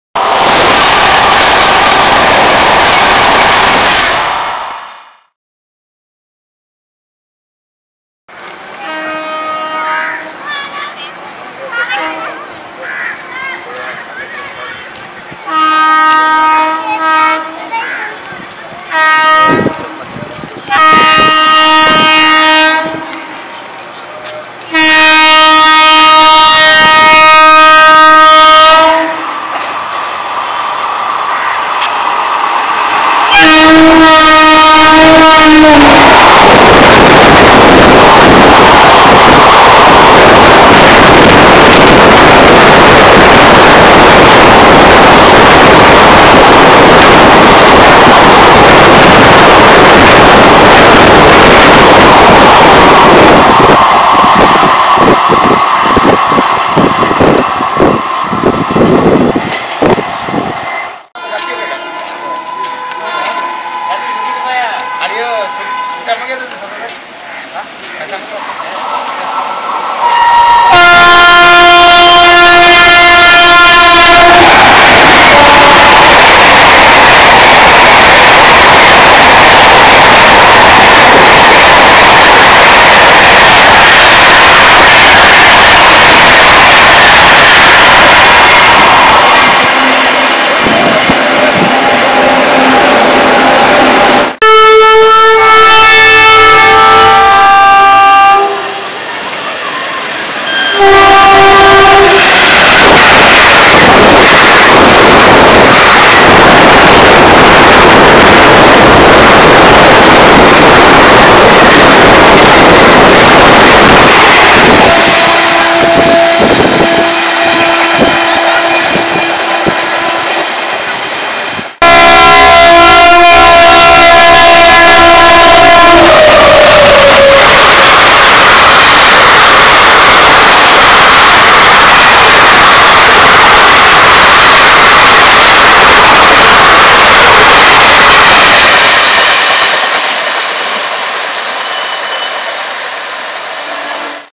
madly honking wap4 continuously blinks flasher light II coalfield express + bagh express I P7 IGBT
130km/h storm at Talit by Dhanbad Howrah Coalfield Express
Speedy actions by Sealdah Amritsar Jallianwala Bagh express with 30308 Sealdah IGBT equipped WAP-7
Howrah Siuri Hool Express emergency light blink at a very high speed
Kathgodam Howrah Bagh express